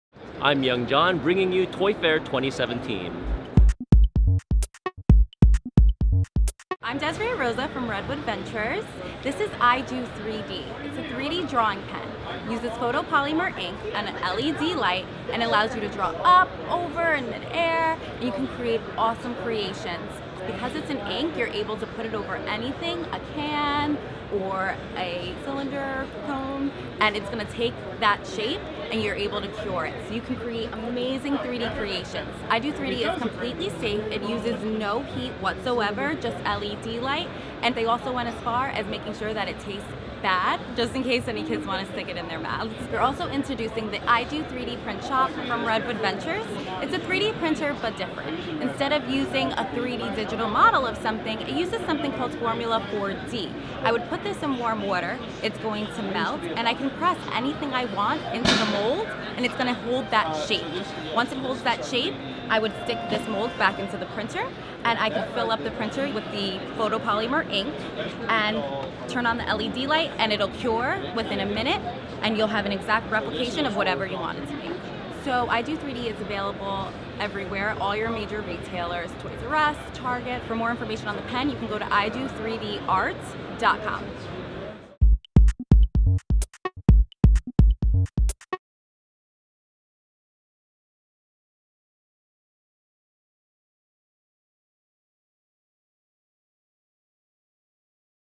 No-Software 3D Printer and Pen, Re-Usable Molds
"Draw" over anything to make whatever shapes you want with the "I Do 3D" pen. It uses no heat for safety (light only) with the ink tasting like crap - to dissuade any child from eating the goods.

And forget the complexity involved with 3D model software - the "I Do 3D Printer" uses a fun (and re-usable!) analogue mold that you can shape and cure into anything you wish. As long as it's not bigger than your thumb...

Produced by: Young Jeohn
With: OtakusAndGeeks.com

Toy Fair Event (02/17)
@Jacob Javits Center, New York City